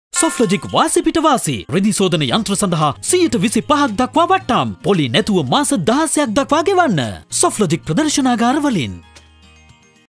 සොෆ්ට්ලොජික් වාසි පිට වාසි! - Washers
සොෆ්ට්ලොජික් වාසි පිට වාසි!
රෙදි සෝදන යන්ත්‍ර සඳහා 25%ක් දක්වා වට්ටම්. මාස 16ක් දක්වා පොලී නැතුව ගෙවන්න ගන්න.

අදම සොෆ්ට්ලොජික් හෝ සොෆ්ට්ලොජික් මැක්ස් ප්‍රදර්ශනාගාරයක් වෙත හෝ වෙත පිවිසෙන්න.
කොන්දේසි සහිතයි.